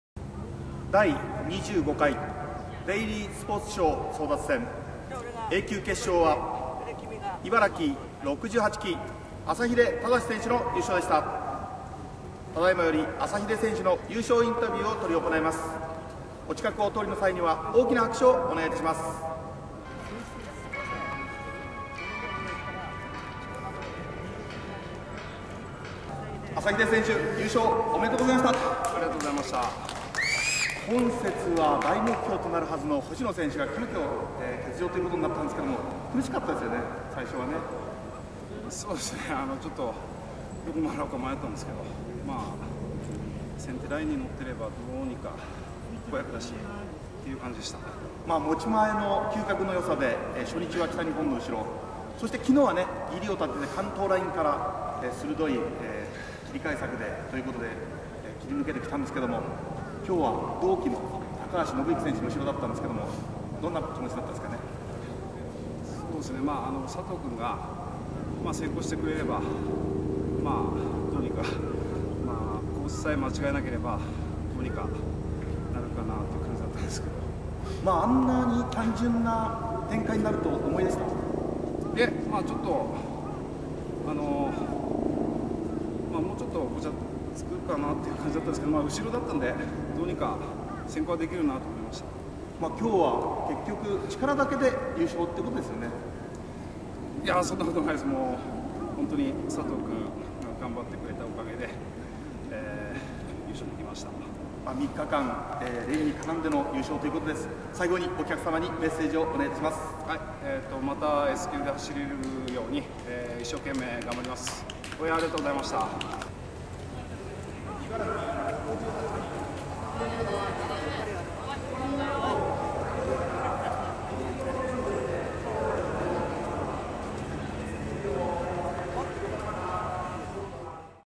2013.03.06 大宮競輪 A級決勝10R 優勝者インタビュー
第25回デイリースポーツ賞争奪戦　F1